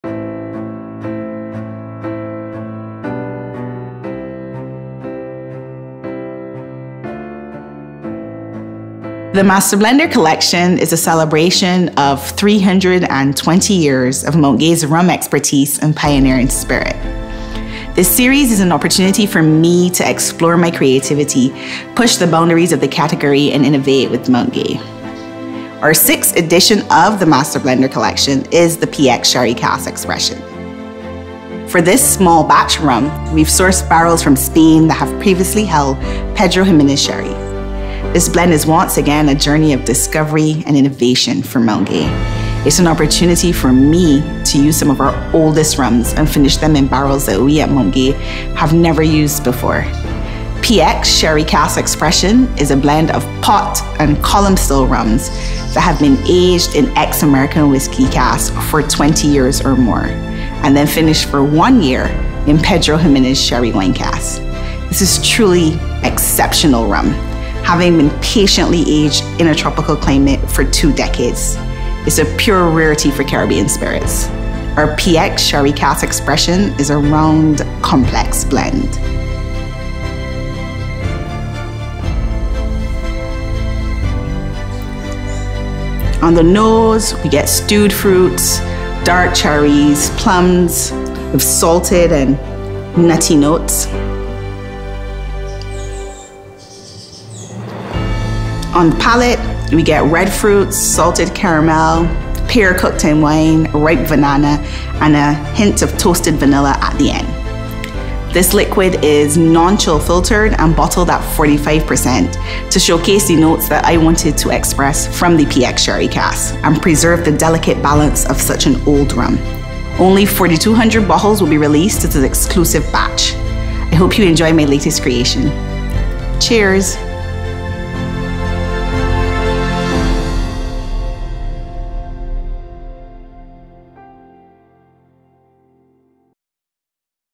Master Blender PX Sherry Cask Expression
Introducing PX Sherry Cask Expression: The Sixth Release of our Master Blender Collection! Trudiann Branker and her team unveil an exclusive blend crafted from the finest and oldest rum stocks in our Mount Gay bonds. This small batch rum, meticulously created in the time-honored Mount Gay house style, marries the artistry of pot and column still rums.

For two decades, these exceptional spirits aged in ex-American whiskey casks, basking in the Barbadian climate where the marriage of heat and humidity amplifies their interaction. The result? A tropical ageing process that bestows deeply aromatic flavors of burnt oak and toasted vanilla in a complex aged spirit.

But, our quest for perfection does not end there. Trudiann and her team added an additional year of maturation in Pedro Ximenez Sherry wine casks, elevating this exclusive blend to new heights.

Available soon at selected retailers worldwide.